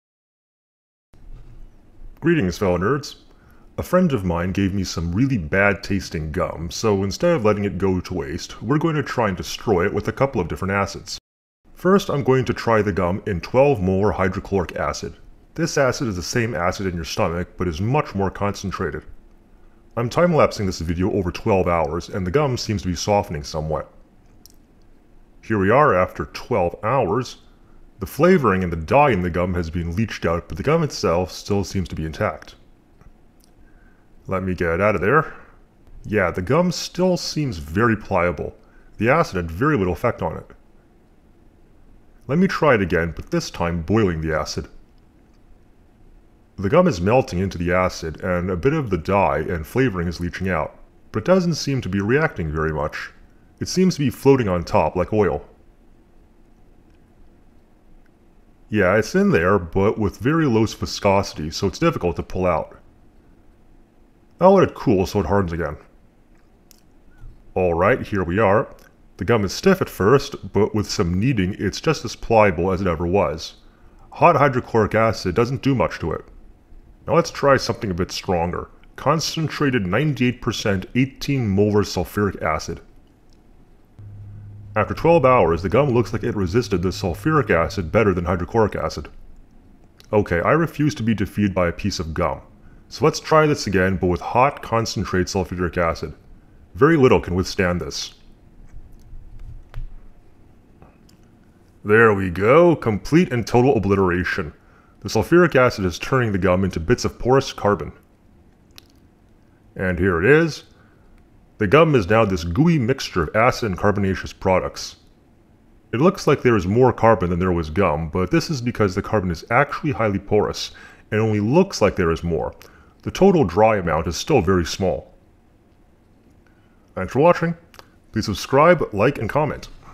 Gum in Acid
In this video we put gum in hydrochloric acid and sulfuric acid and see what happens